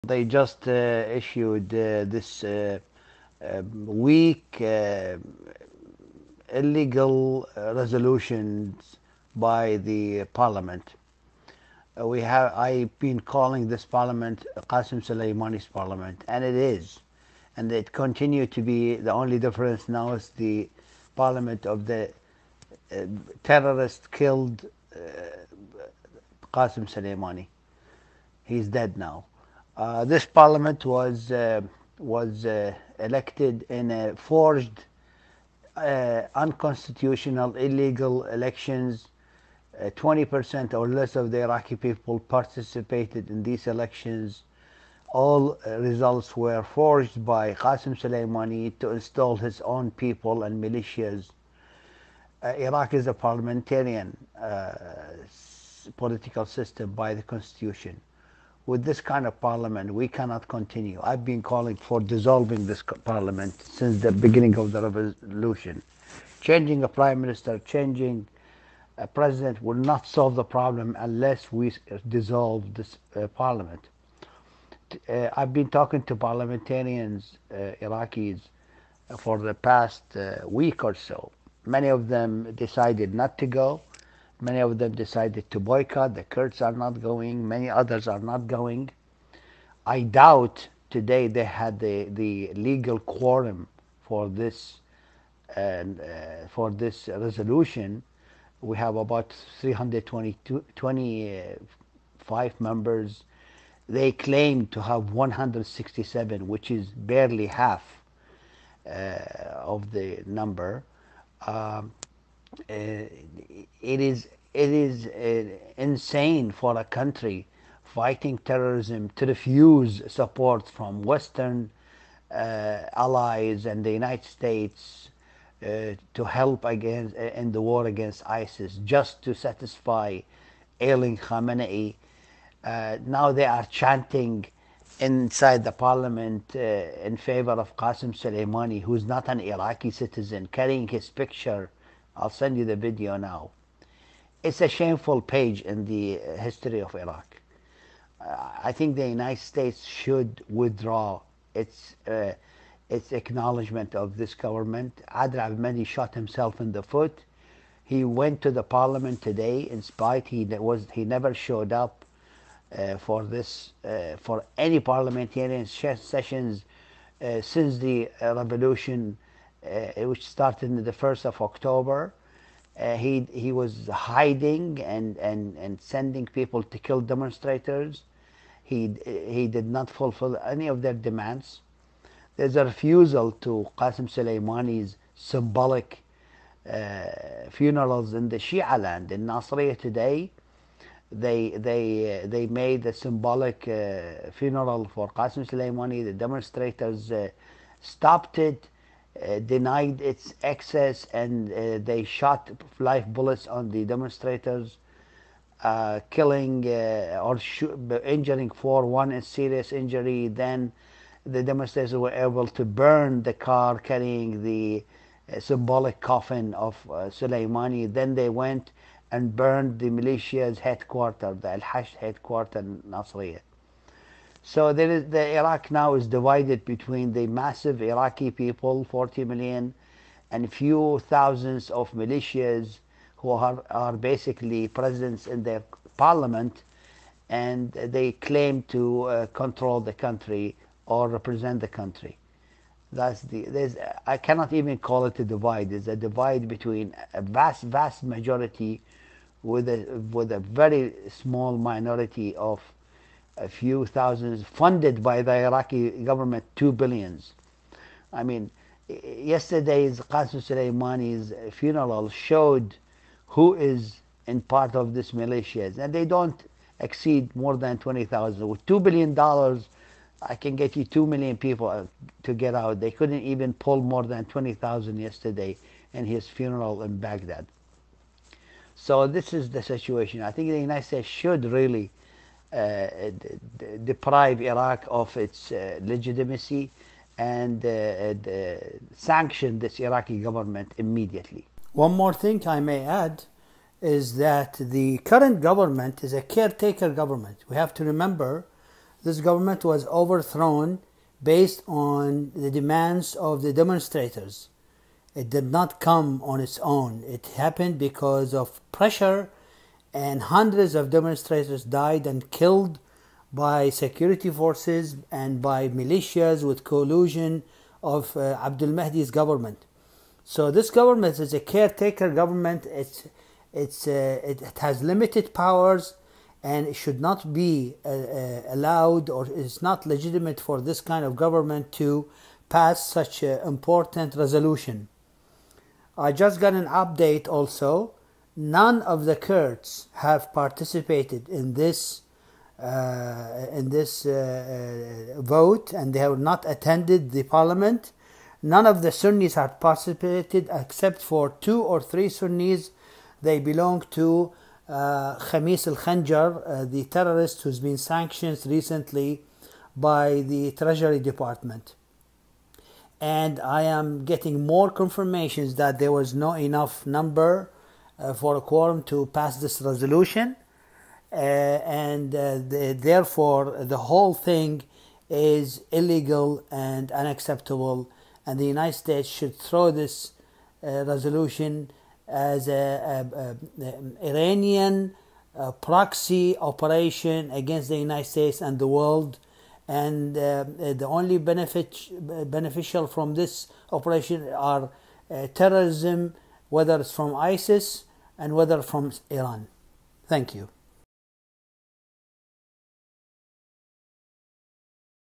Entifadh Qanbar Today’s resolution passed by the Iraqi Soleimani Parliament is illegal 5 Jan 2020
Entifadh Qanbar
Today’s resolution
passed by the Iraqi Soleimani Parliament
is illegal 5 Jan 2020